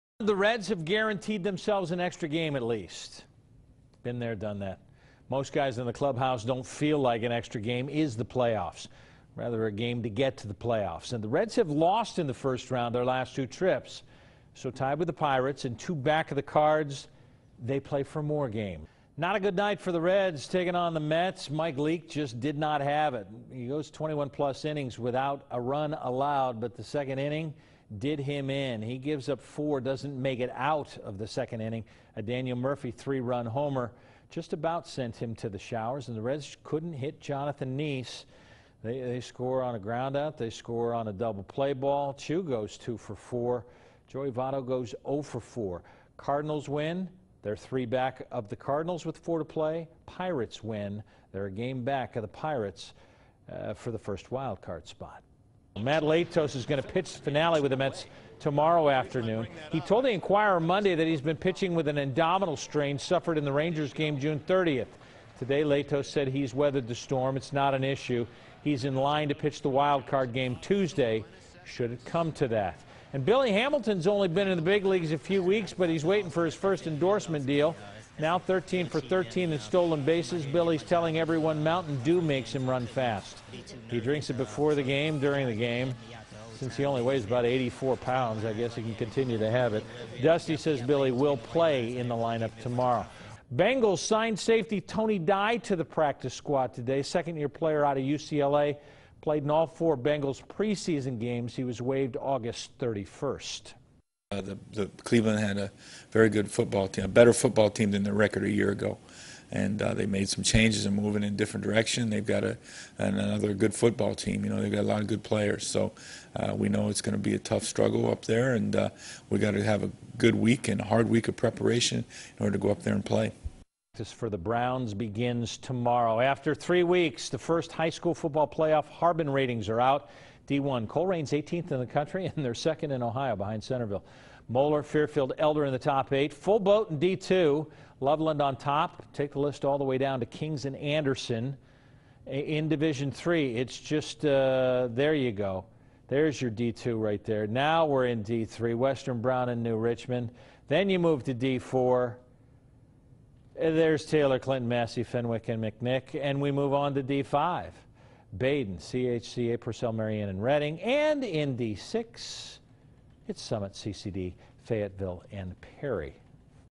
Daily Sportscast: Tuesday, September 24
Brad Johansen has Reds, Mat Latos, Billy Hamilton, Bengals and high school football